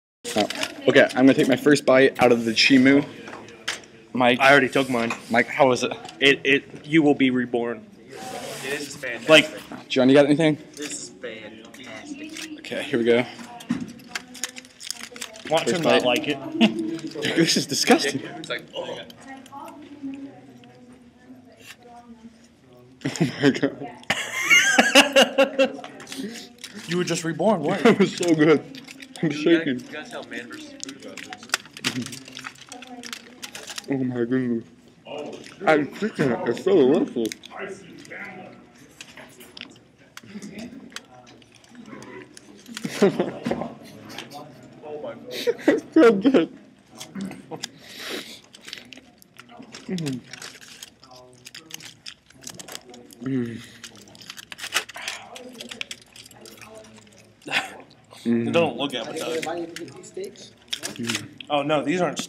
Treehouse Truck - Chi Moo
The first bites out of a Chi Moo, that we got from Treehouse Truck. Follow @TREEHOUSETRUCK on twitter to find their locations!